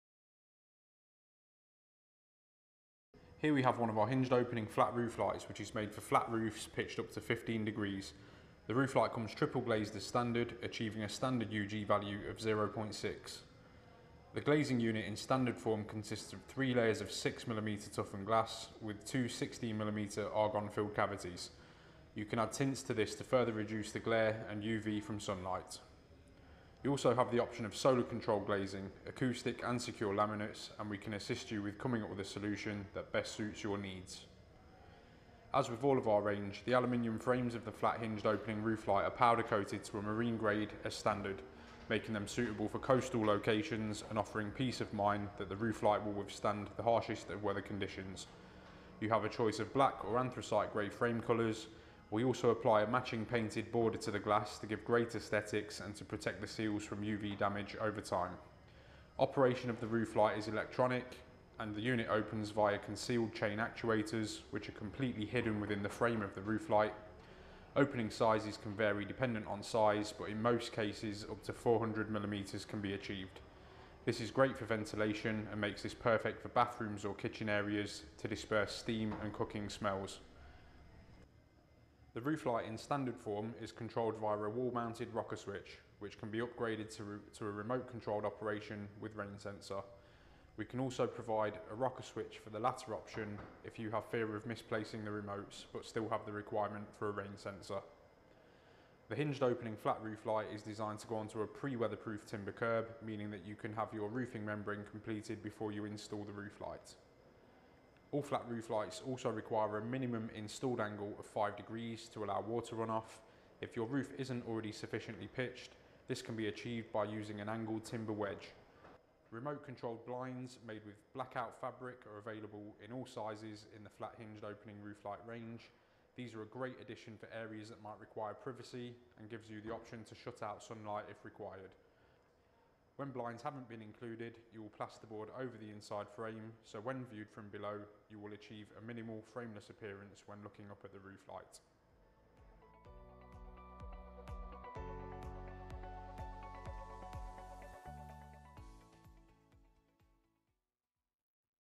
Hinged Opening rooflight from Roof Maker
Designed and handcrafted to the highest standards in our UK factory, the Roof Maker Hinged Opening rooflight is perfect for rooms where you need a high flow of fresh, ventilating air, such as busy kitchens or steamy bathrooms.

Unlike most rooflight companies, we build to your exact specification and don’t charge extra for bespoke sizes. Our frame sits outside your aperture, so you’ll see more glass and enjoy more light for your money.

Key Features

– electronically operated by a rocker switch
– triple glazed as standard
– Jet black (RAL 9005) or Anthracite Grey (RAL 7016) frames
– Ug-values as low as 0.6w/m2k
– 20-year unit seal warranty
– unit seals 80% deeper than the industry standard
– easy clean glass

Optional upgrades

– automatic rain sensor and remote control
– choice of specialist glass types
– different glass tint colours
– remote controlled electric blackout blinds